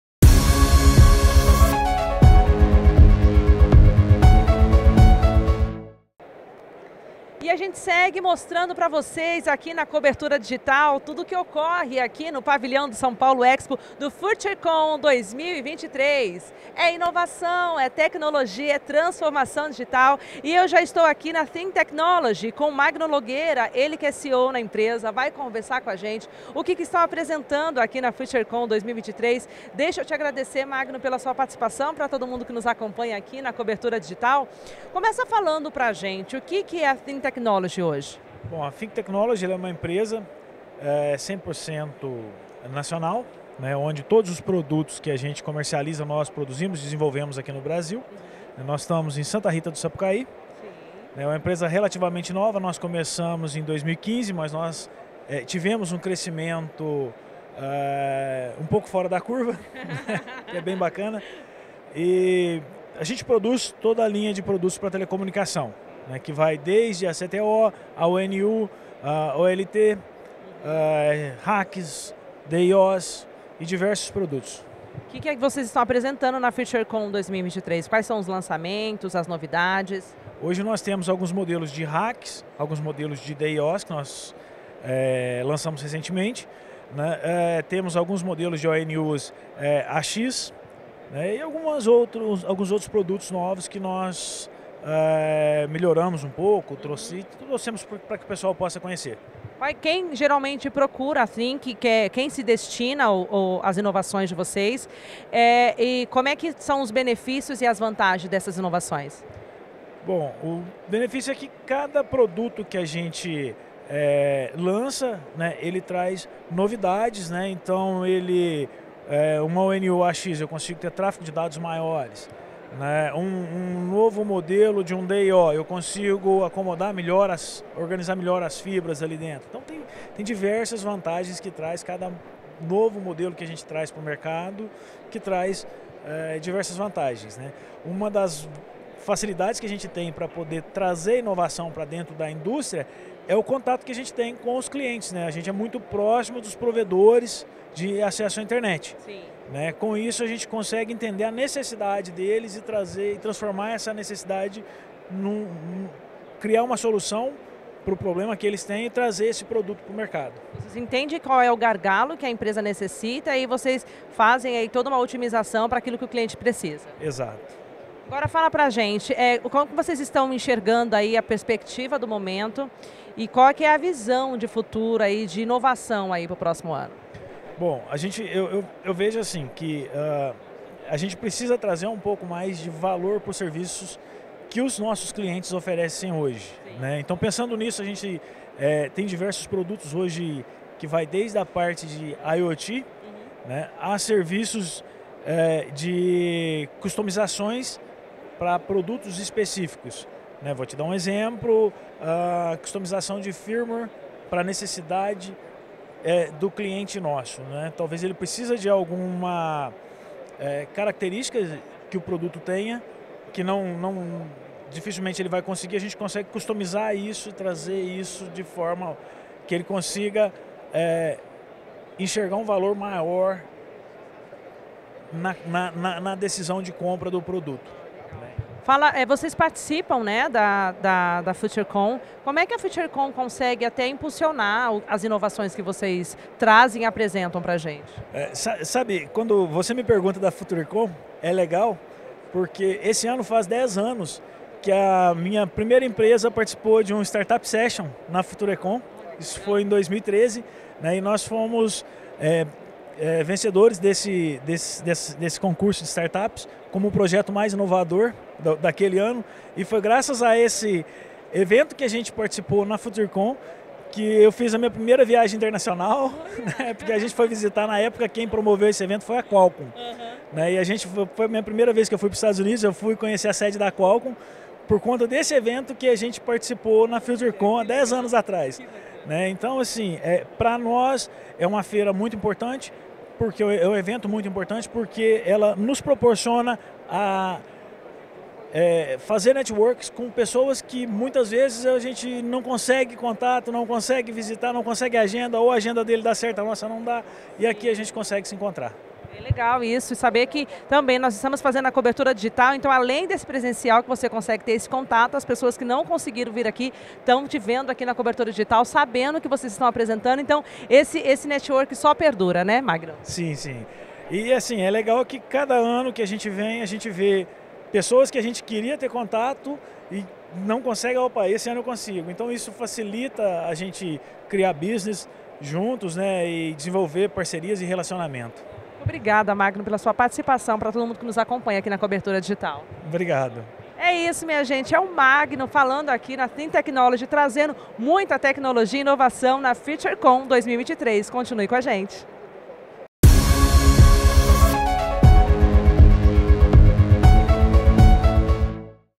Think Technology no Futurecom 2023
O Futurecom é a maior plataforma B2B de conteúdo e relacionamento voltada ao universo de conectividade da América Latina. Há mais de 20 anos fomentando o mercado, combinando exposição, experiências, debates, networking e demonstrações sobre os impactos das aplicações de tecnologias em praticamente todos os segmentos da economia, gerando grandes oportunidades de negócios.

Confira o que nossos expositores tem a dizer sobre a edição 2023!